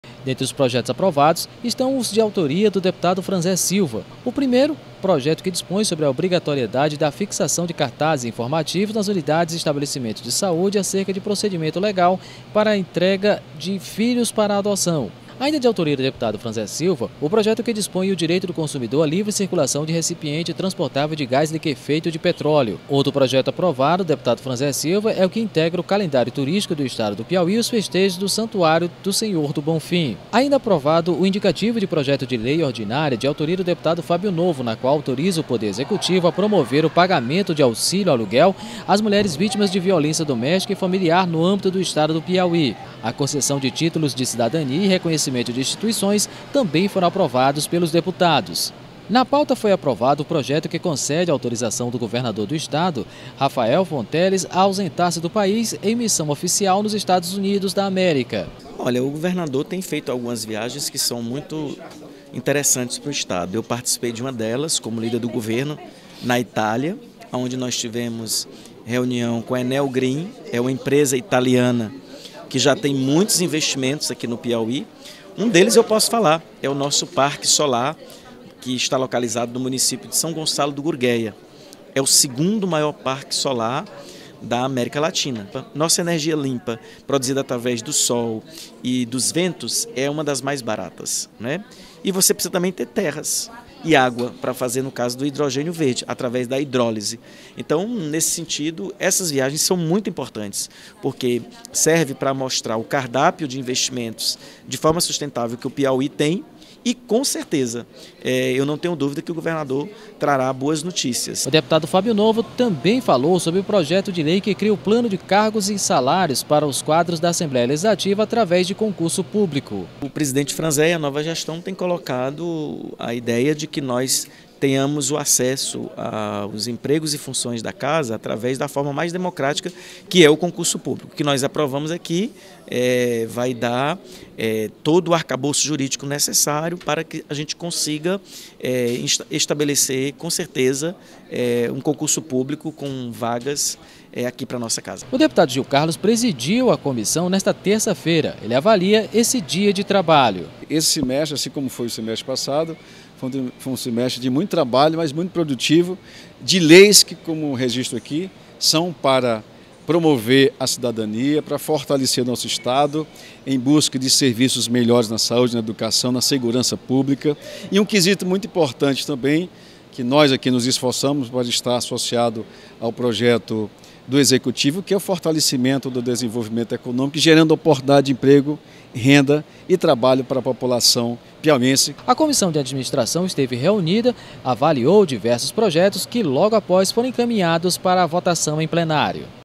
CCJ aprecia projetos no primeiro semestre
Na primeira reunião do segundo semestre da Comissão de Constituição de Justiça da Assembleia do Piauí, projetos do Poder Executivo e de iniciativas parlamentares foram apreciados pelos membros da comissão. Veja na reportagem de James Almeida e Volmar Cardoso, da TV Assembleia.